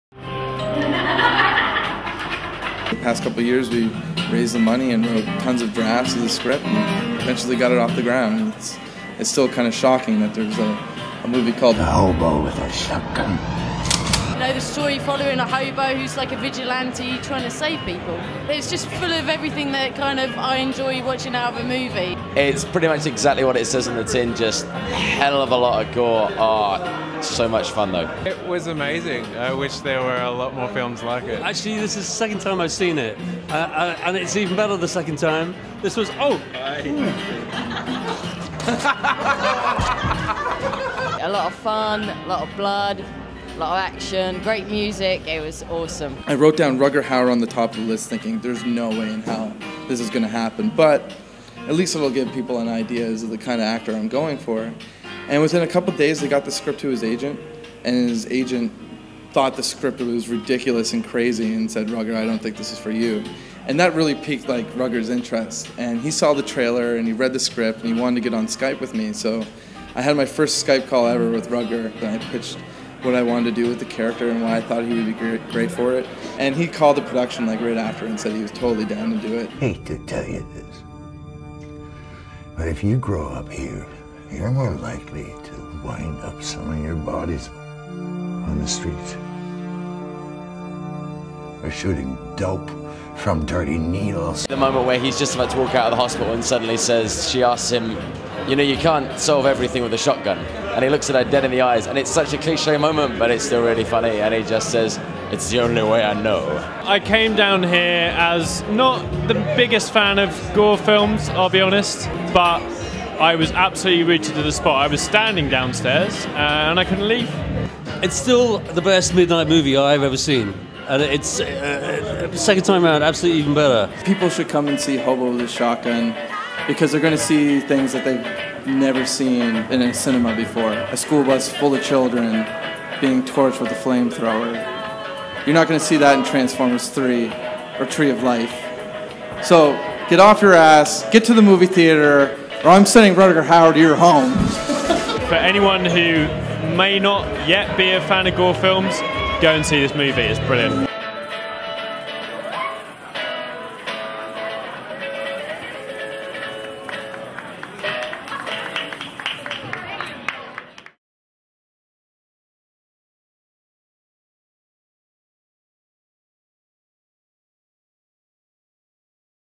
Hobo London Premiere
Jason Eisener visits London for the Premiere and Launch party of Hobo With a Shotgun.